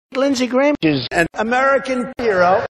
Donald Trump: Lindsey Graham is an American hero - Mishmasher
Donald Trump will say anything… that you want him to!

Mishmasher lets you easily make and share awesome mashups.

Full transcript: Lindsey Graham is an American hero